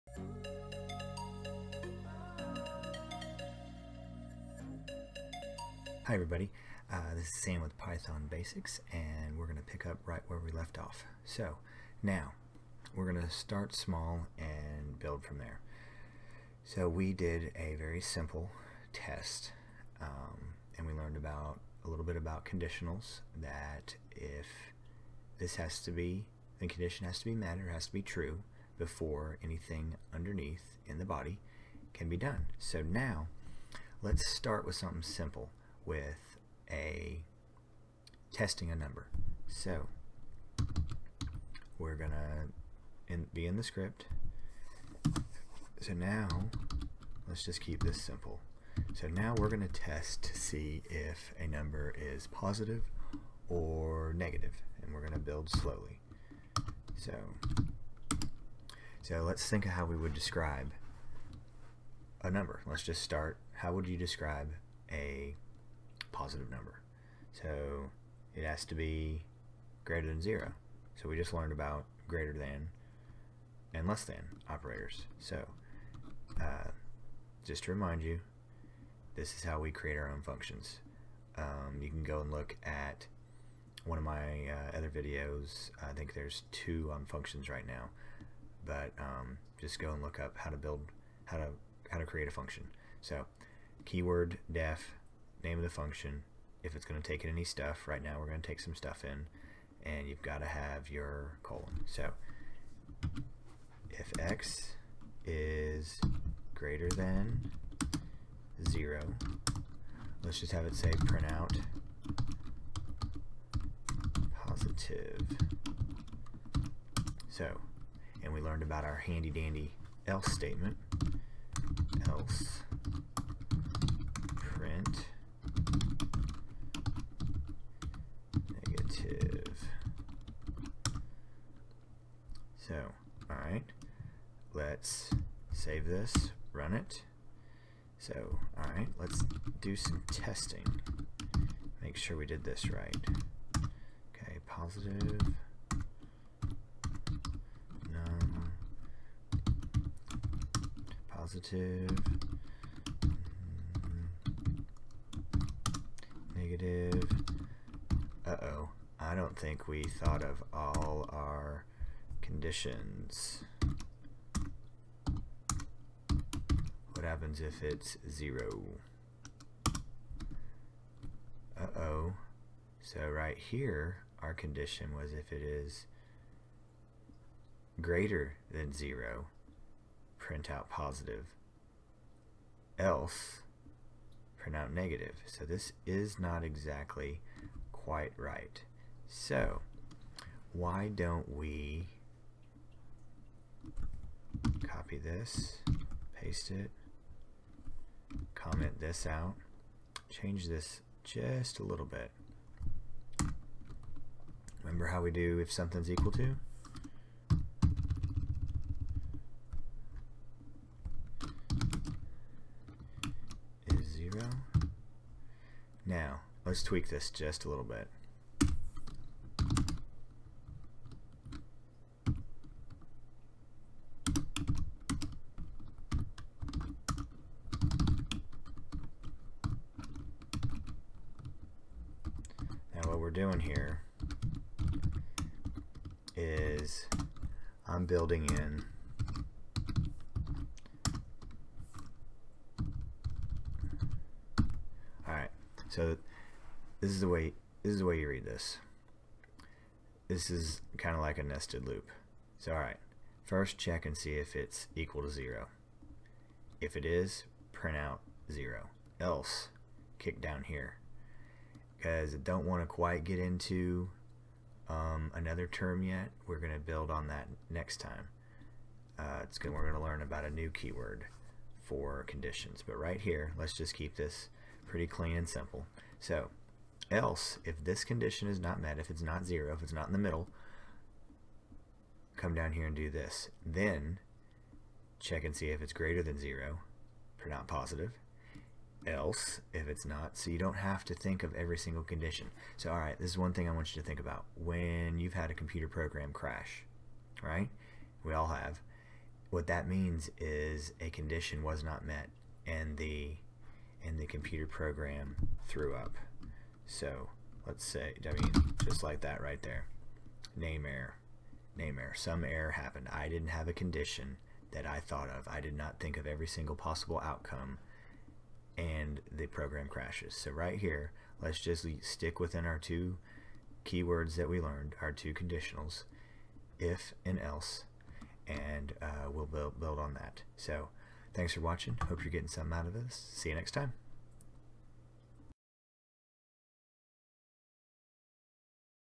Python Basics Conditional Statements nested if - else pt 3
Learn about and how to use nested if else statements.